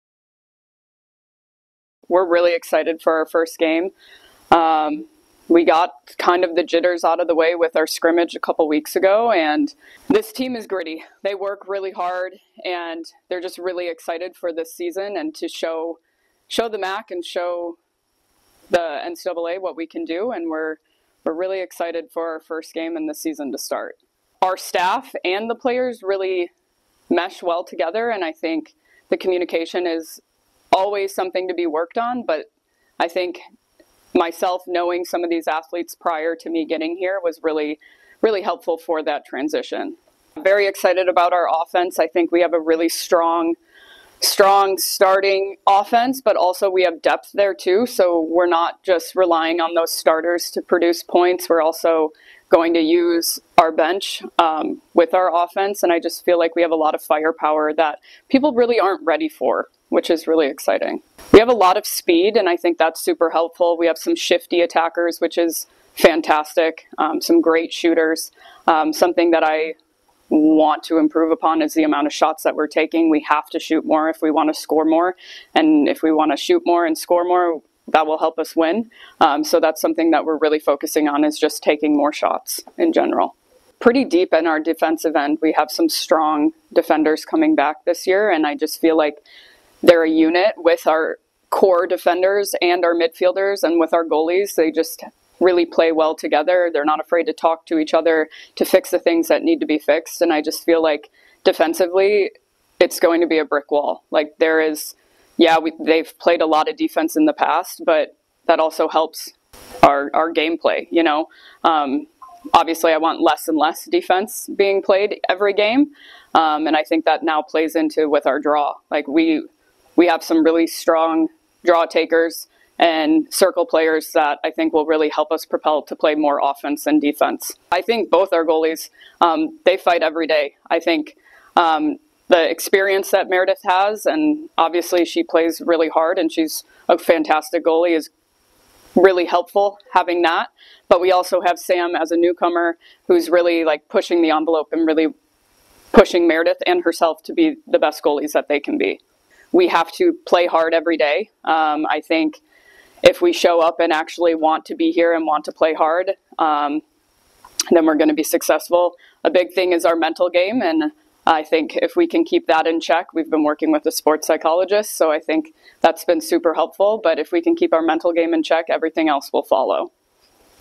Detroit Mercy Women's Lacrosse 2024 Preview
Head coach Madeline Dugan previews the 2024 season.